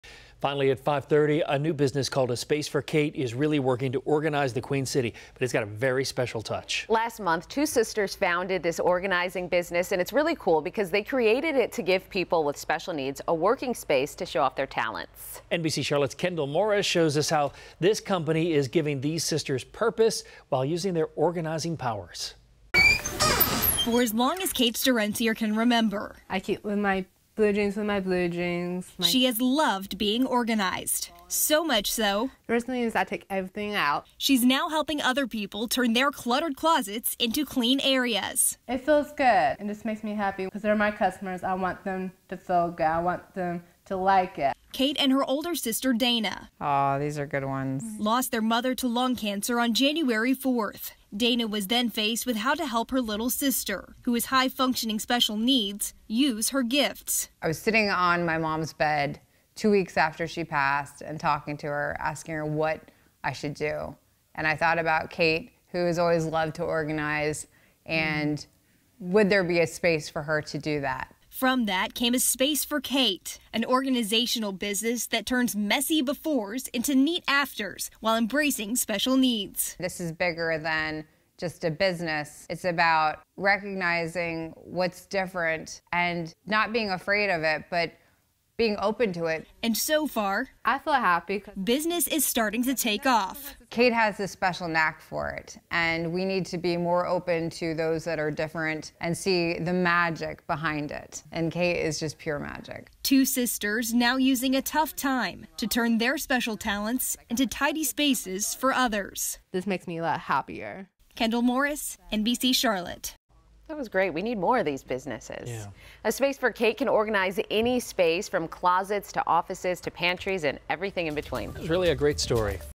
'Space for Kate' gives local sisters purpose
Last month, two sisters founded the organizing business. They created it to give people with special needs a working space to show off their talents.